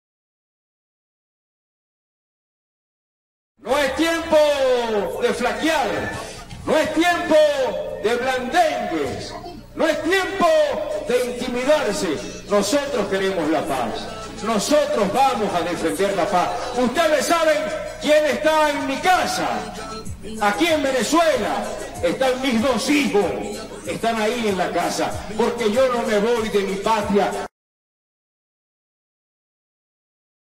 SE FUE LA LUZ|| EN LA TARIMA DEL CONCIERTO DEL CHAVISMO EN LA FRONTERA
En medio del concierto del chavismo "Hands off Venezuela", este viernes 22 de febrero, Winston Vallenilla, durante un discurso en el que expresó su lealtad irrestricta a Nicolás Maduro, se fue el servicio eléctrico en el puente Tienditas, del lado del estado Táchira. El director de la transmisión desvió la atención del hecho enfocando al público asistente.